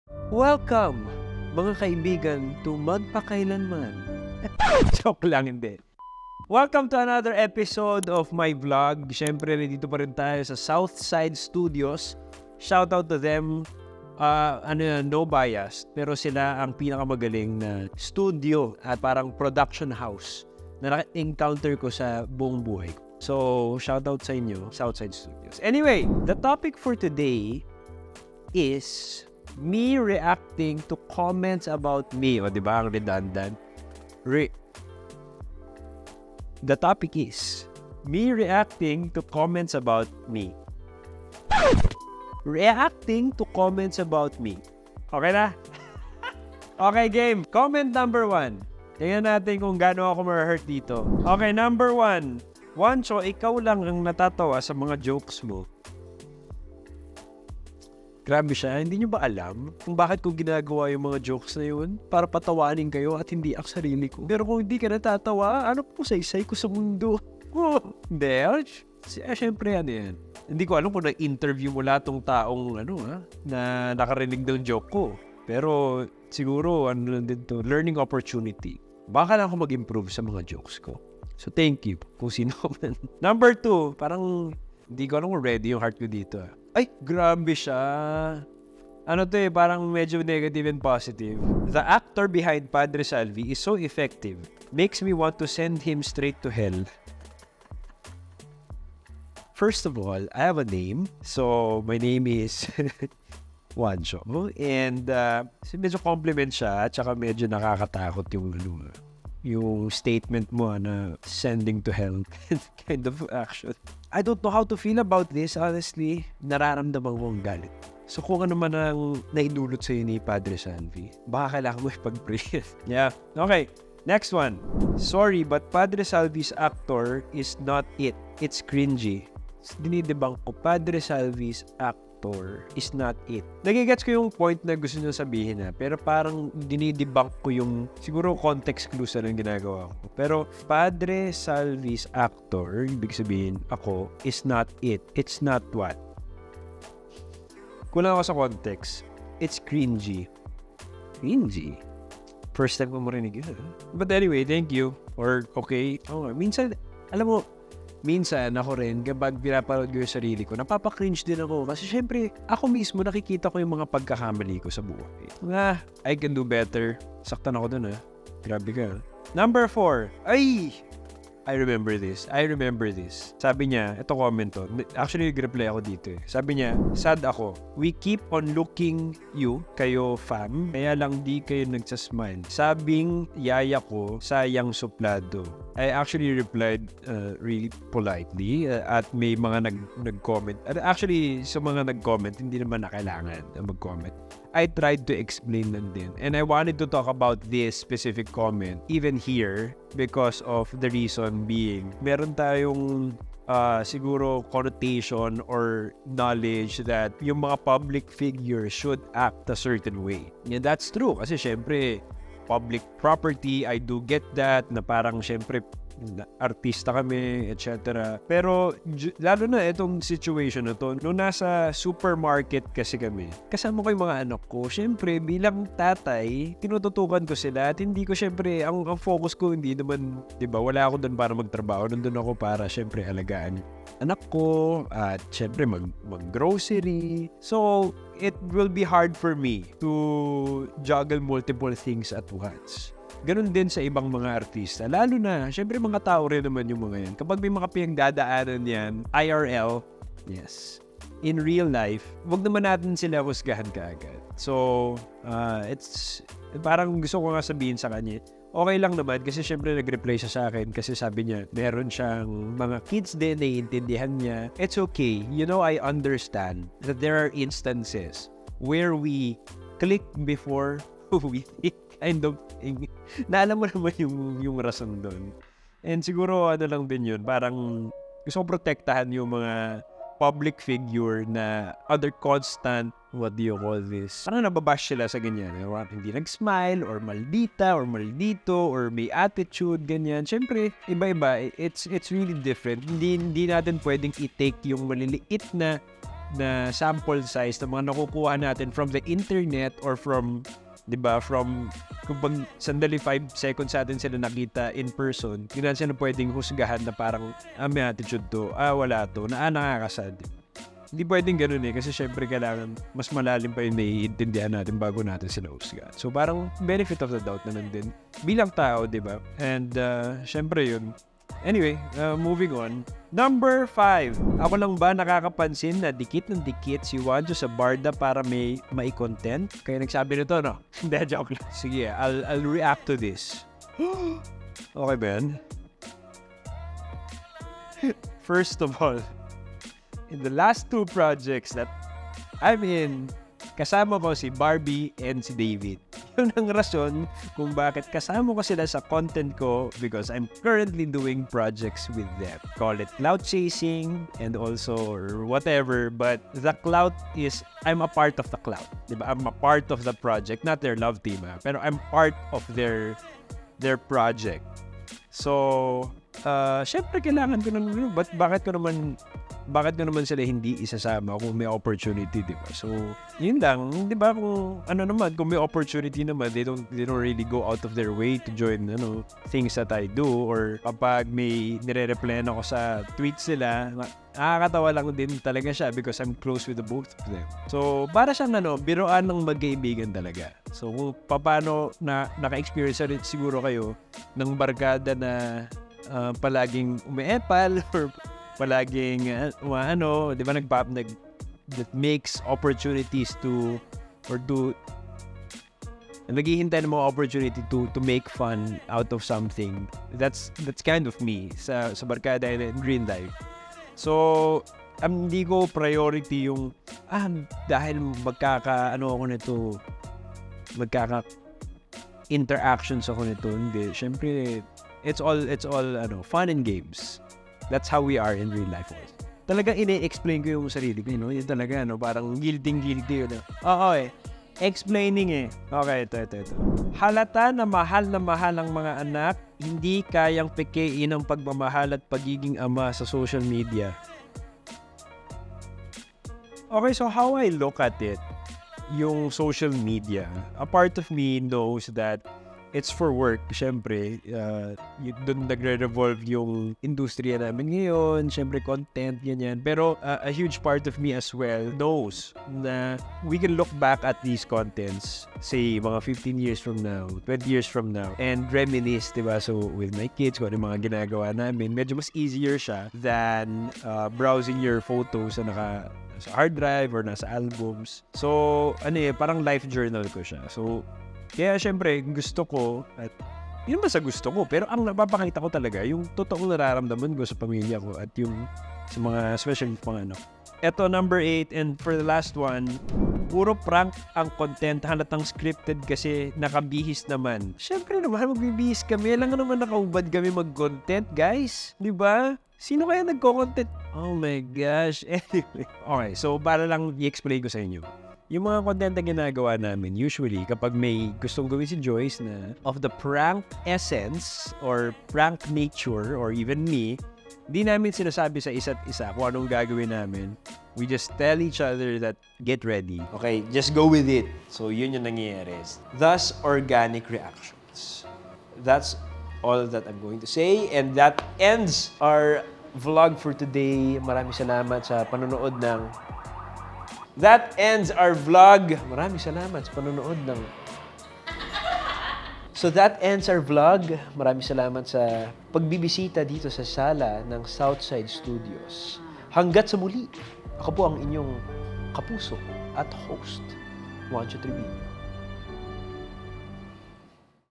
Reacting to Comments About Me 👀 (Suplado? Ginagamit ang BarDa?) | Juancho Trivino
Walang natatawa sa jokes ko? Suplado daw ako? Ginagamit ko lang daw ang BarDa (Barbie Forteza and David Licauco) for content? Lahat ng tanong na gusto niyong malaman, nasa bidyo na ito.